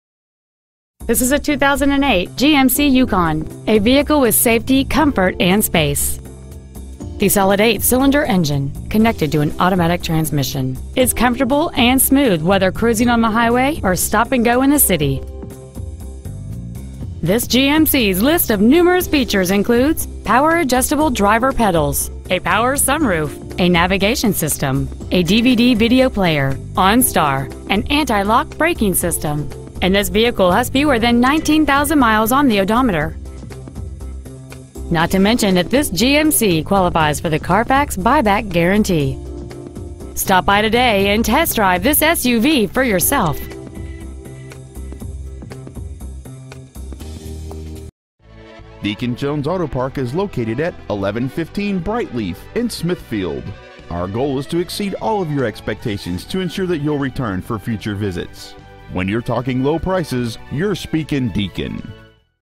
2008 GMC Yukon Garner NC
Year: 2008
 Make: GMC
 Model: Yukon
 Engine: 6.2L 8 CYL. SEFI
 Trans.: Automatic
 Exterior: White
 Miles: 18,730
 Interior: Brown / Tan

 Preowned 2008 GMC Yukon Smithfield NC